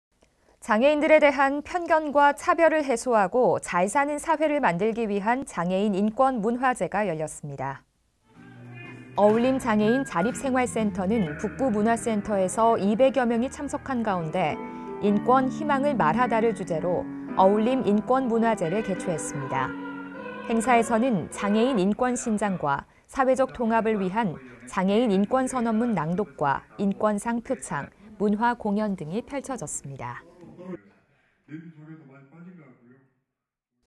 220617 북구 장애인 인권문화제
■ '장애인 인식 개선' 제9회 어울림인권문화제 개최

장애인들에 대한 편견과 차별을 해소하고
잘 사는 사회를 만들기 위한
장애인인권문화제가 열렸습니다.

어울림장애인자립생활센터는
북구문화센터에서 2백여 명이 참석한 가운데
'인권, 희망을 말하다'를 주제로
어울림 인권 문화제를 개최했습니다.

행사에서는
장애인 인권 신장과 사회적 통합을 위한
장애인 인권선언문 낭독과 인권상 표창,
문화공연 등이 펼쳐졌습니다.